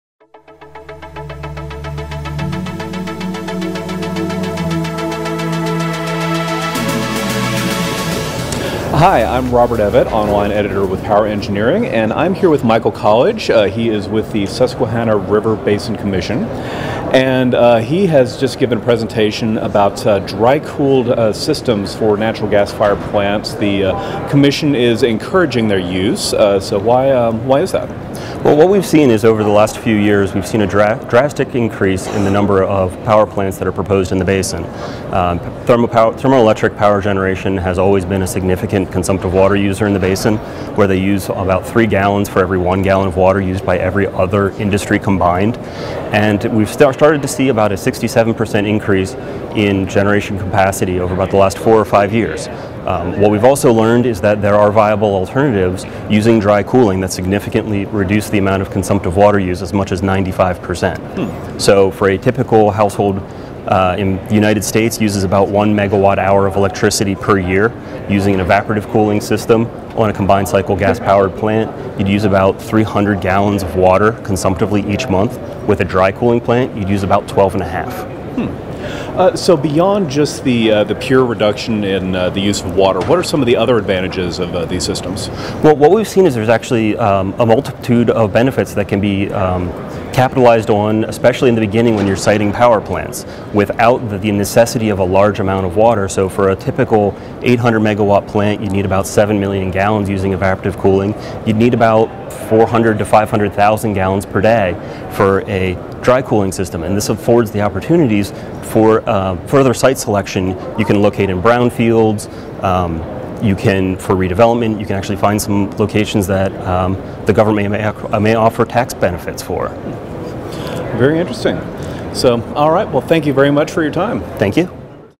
PGI2017 -- Michael College
Interview with Michael College, Supervisor, Surface Water - Project Review at the Susquehanna River Basin Commission. #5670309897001